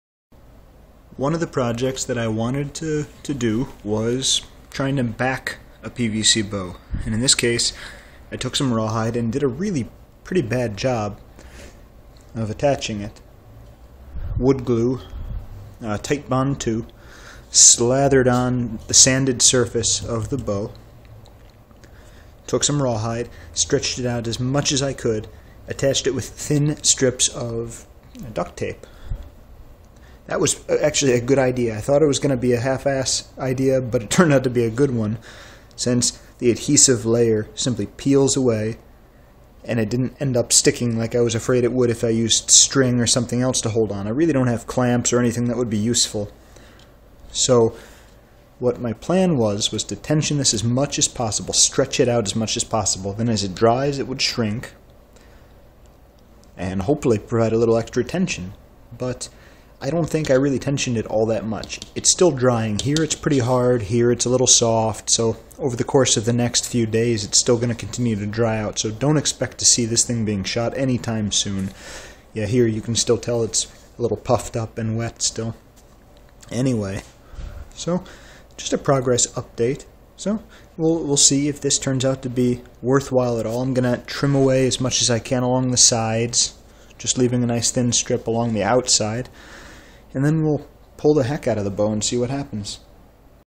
Rawhide backing a PVC Bow
This experiment is still in the early stages. I'm not sure if it's going to hold up, or if it's going to peel off when the bow is fully drawn. Still, I think it's an experiment worth doing.